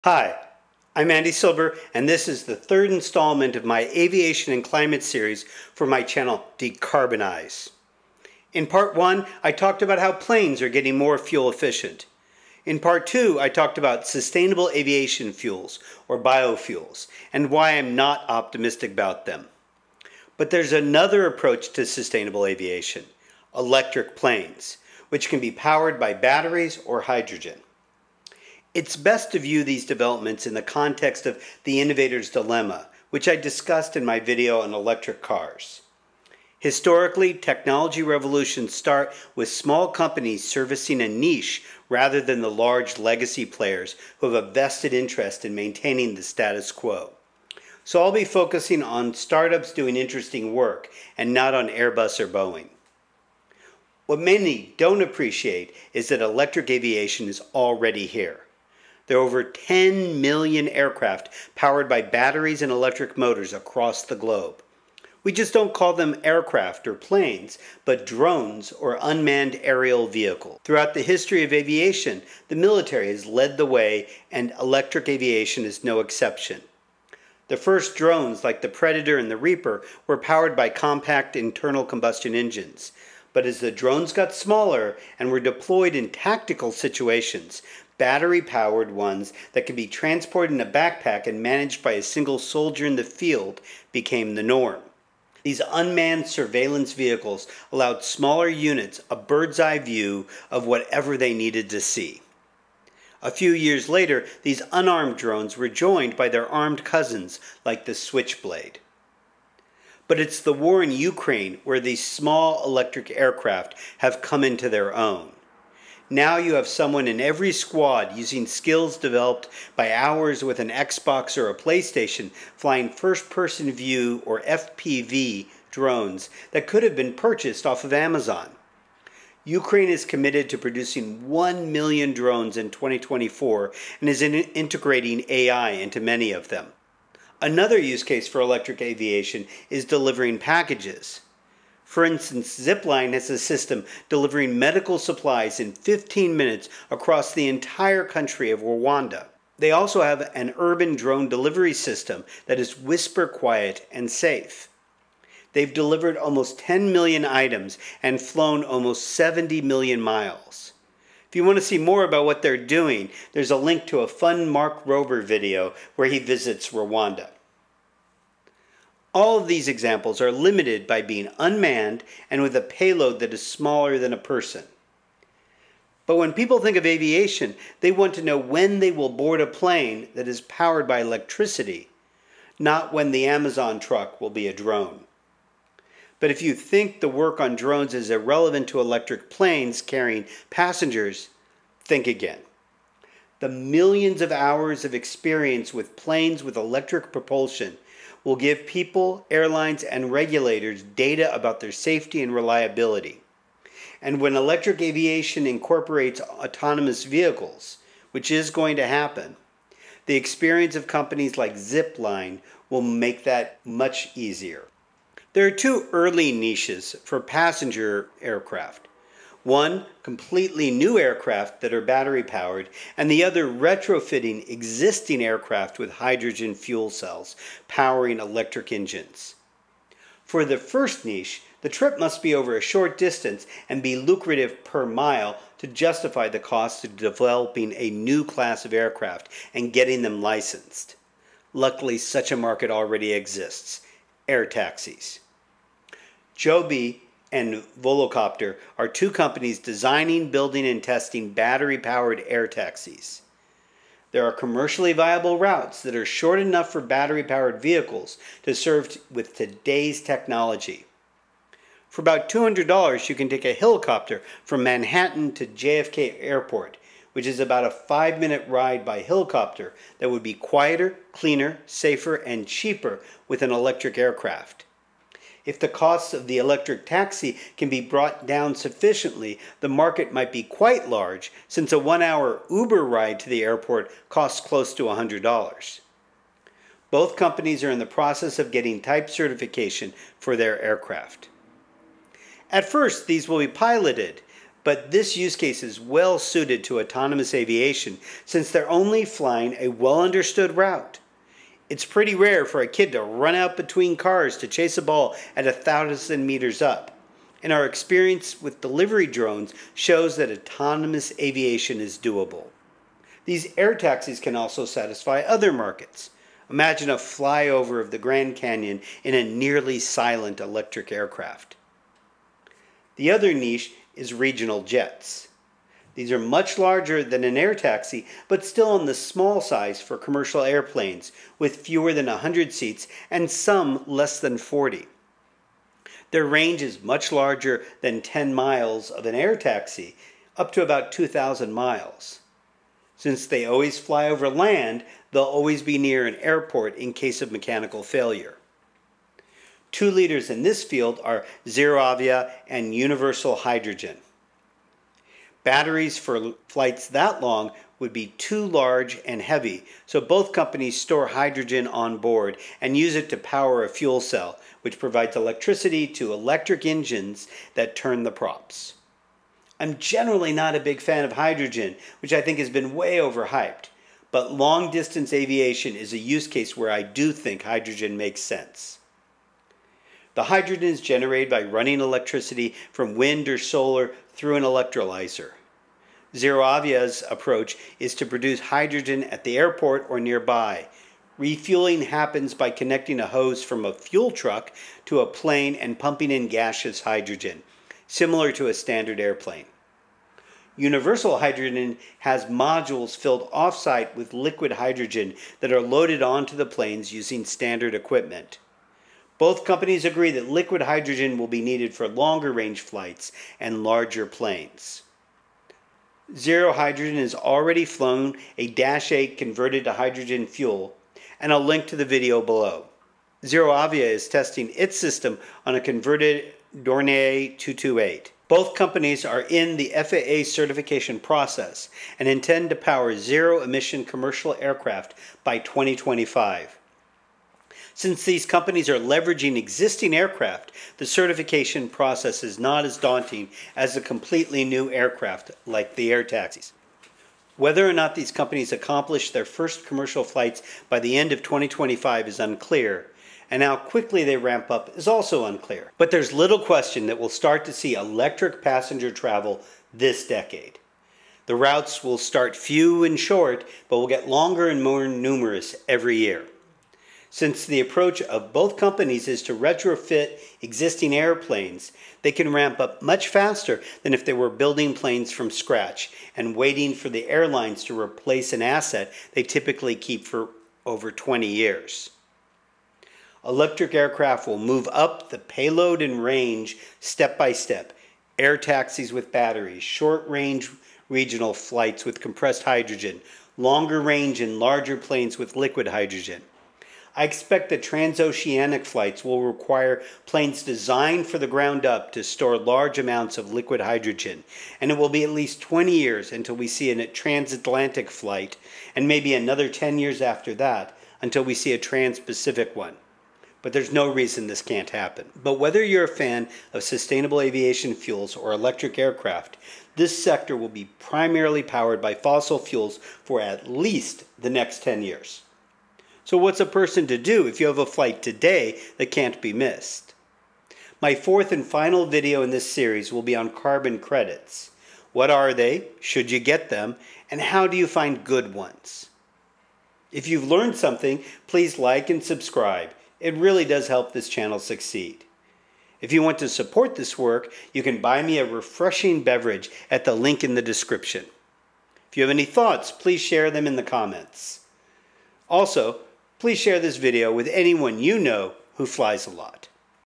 Climate and Aviation III: Electric Aviation
Exciting startups are working on opening up a new world of carbon-free aviation, powered by batteries and hydrogen.

This is part III of the Decarbonize! series on climate and aviation.
Climate and Aviation I: the story of how a fast plane died and a fuel-efficient one was born

Timestamps
Intro 0:00
Drones  0:52
Air Taxis 3:55
Regional Jets 5:24
Conclusion 7:35